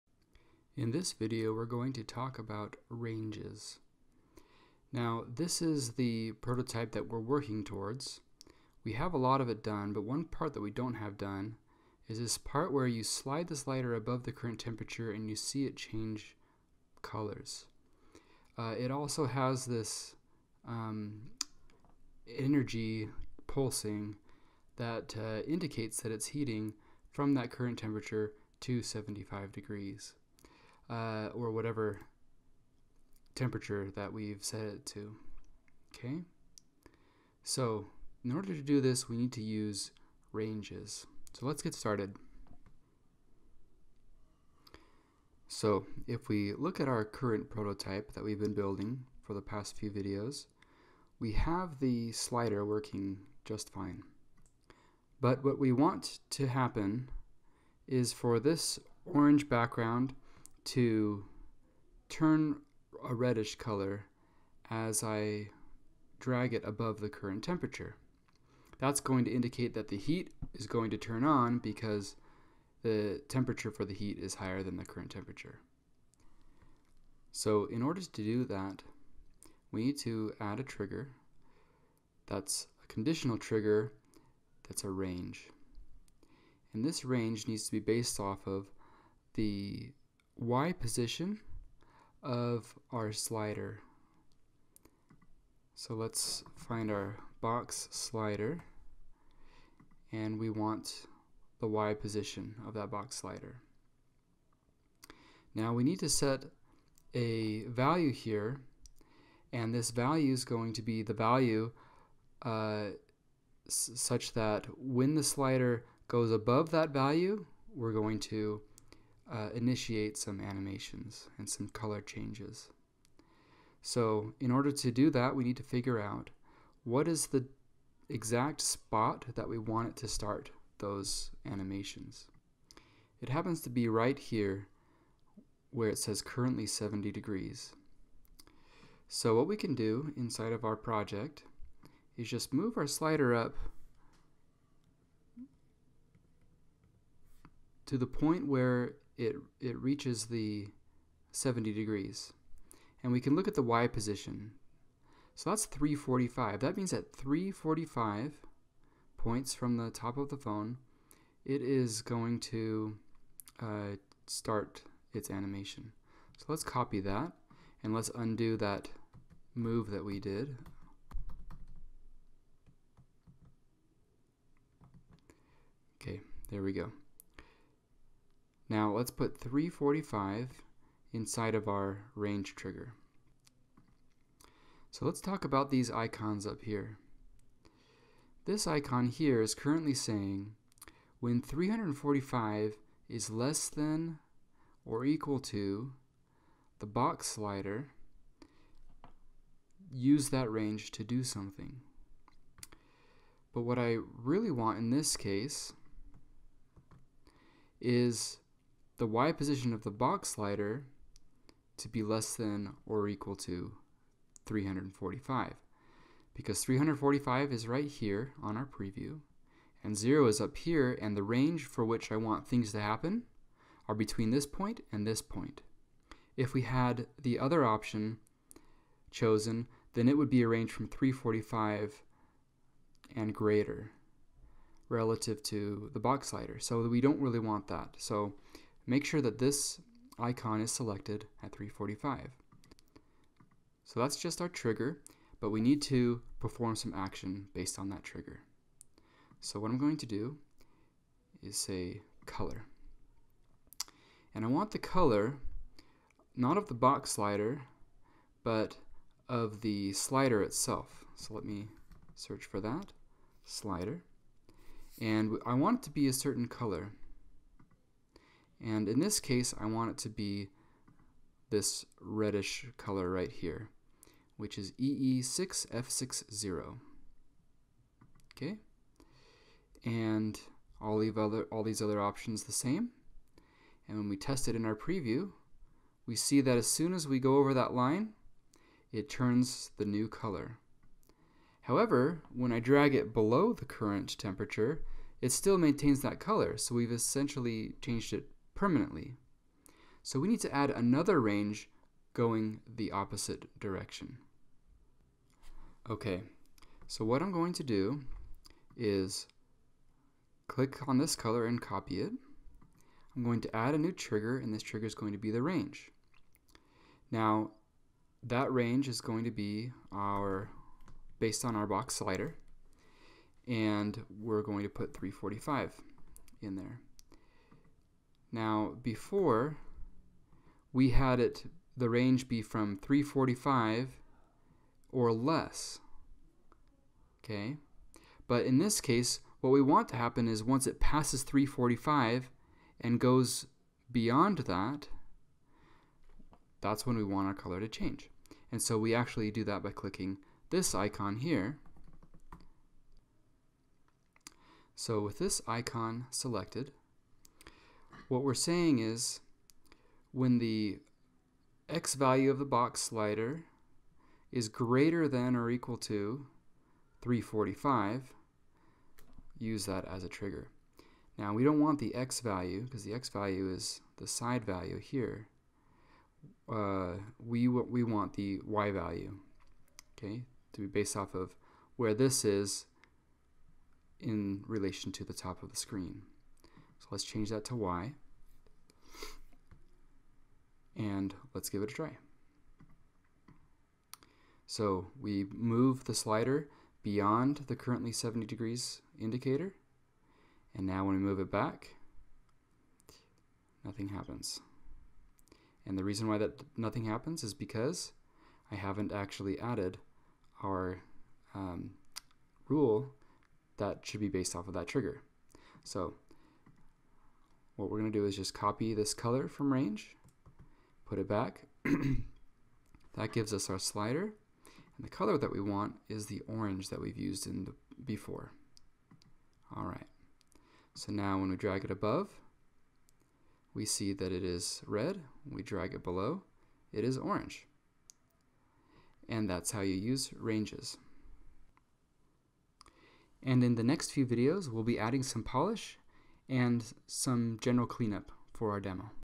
ProtoPie Demo 4 - Ranges
A ProtoPie Demo building a Smart Home Thermostat UI.
Covers Ranges.

This is my first video series, so please forgive the spelling mistakes, umms, repeat phrases, and mumbling.